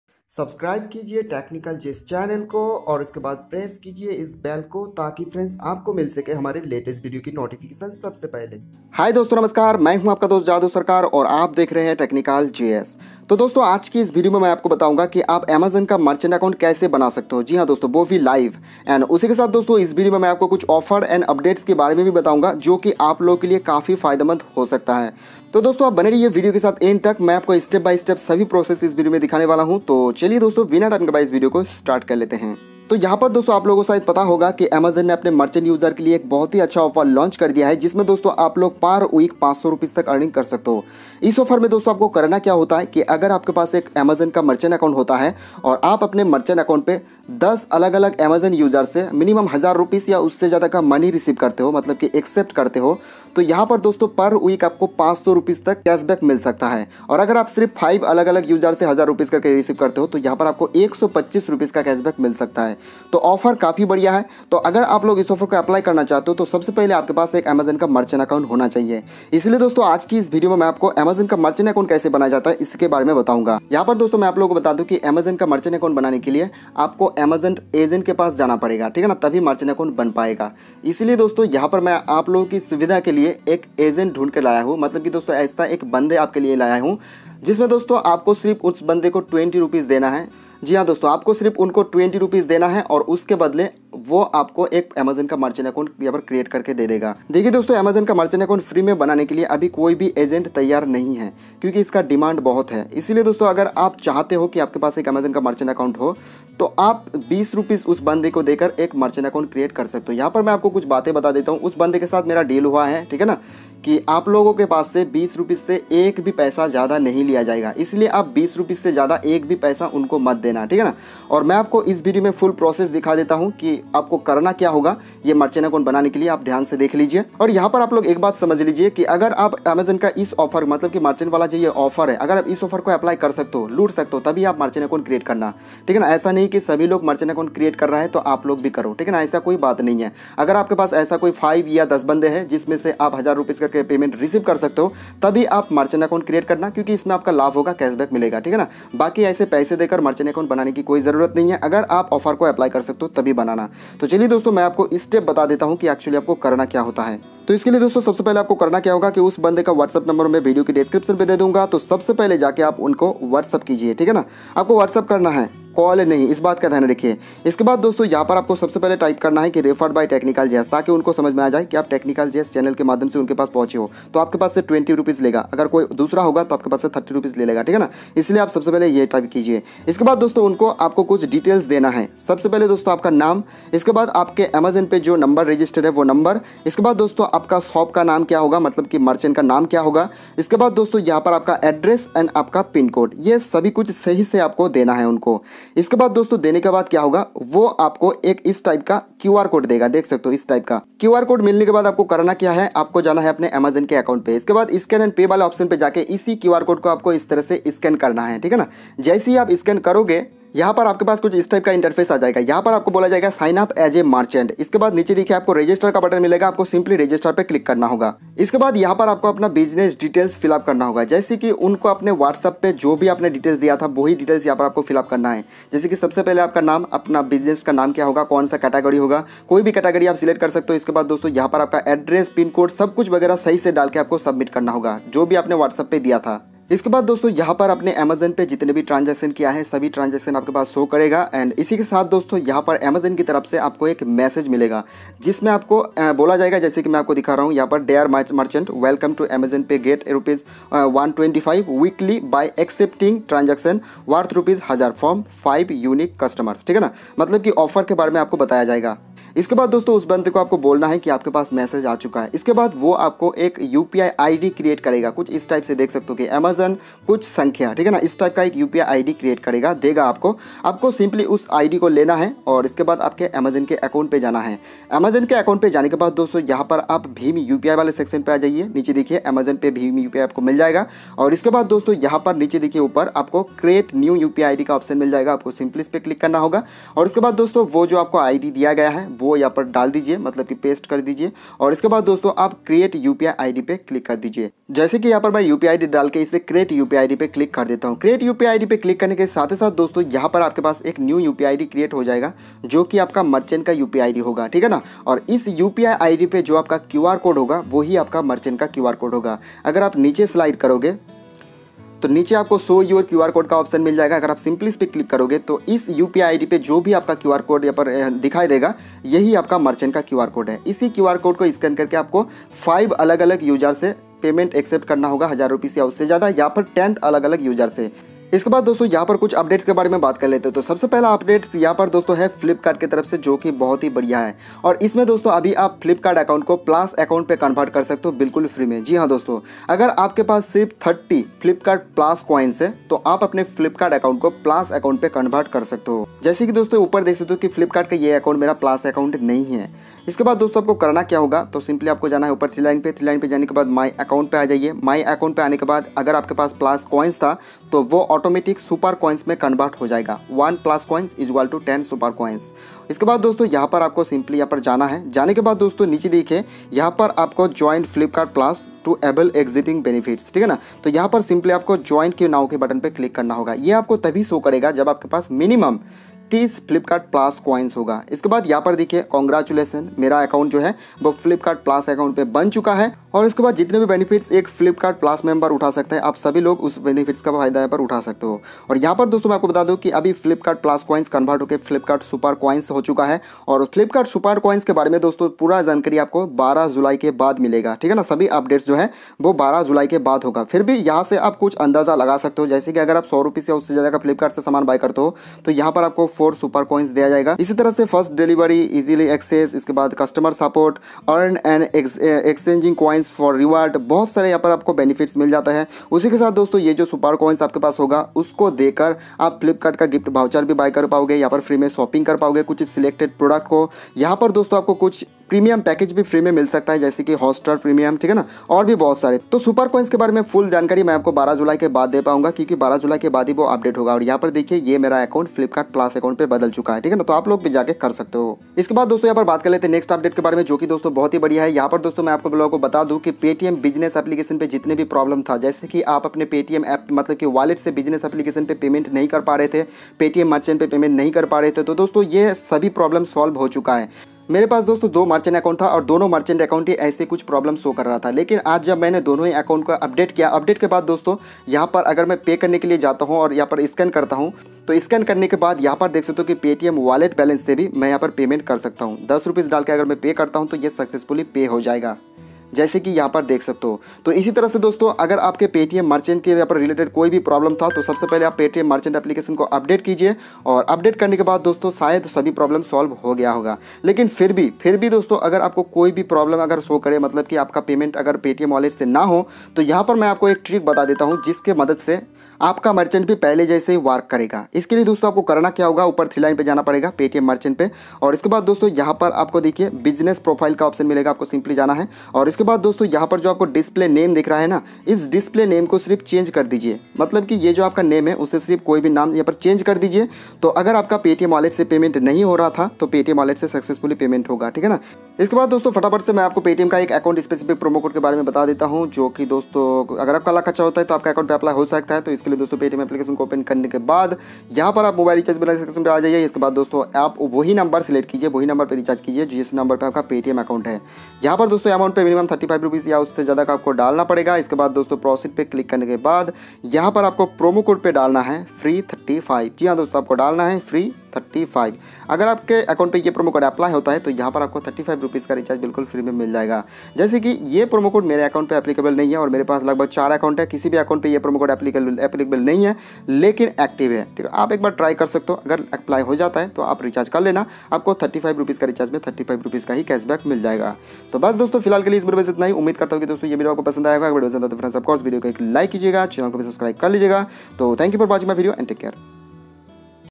How to Create Amazon Merchant Account Live get ₹500, Flipkart Plus Membership, Paytm Merchant Update
Hello friends, In this video I am going to tell you about How to create amazon merchant account live. Amazon merchant ₹500 cashback offer per week. Flipkart new updates, now you comvert your flipkart normal account to flipkart plus account absolutely free. I also tell you about what is flipkart super coins? Benifits of flipkart plus member etc. Paytm merchant all problem solved, now you can use your paytm merchant account normally.

Whatsapp for amazon merchant:-
** expired ( koi new agent ko milega toh number dedunga )

July best offer series:-